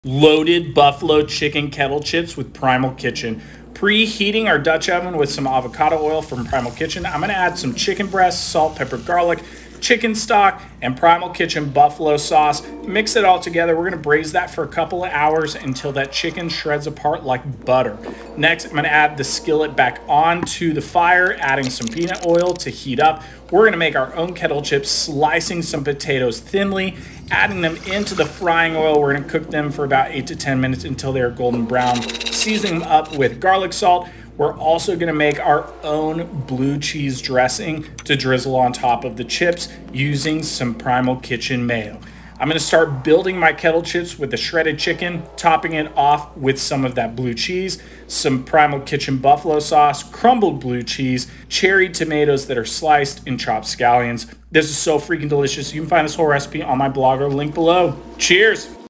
Loaded Buffalo Chicken Kettle Chips 🍗🔥
Loaded Buffalo Chicken Kettle Chips are next level delicious! 🍗🔥

Loaded Buffalo Chicken Kettle Chips are here and they’re ready to satisfy any cheesy, buffalo-y, crispy craving that you’ve got! I partnered up with Primal Kitchen for this recipe and we packed it with flavor from the git go. It started with braising some chicken in stock and buffalo sauce! Then we shredded that chicken up and placed it on top of some homemade kettle chips. Finally, it was served with some blue cheese dressing, halved cherry tomatoes and a drizzle of that delicious buffalo sauce.  🍻 👍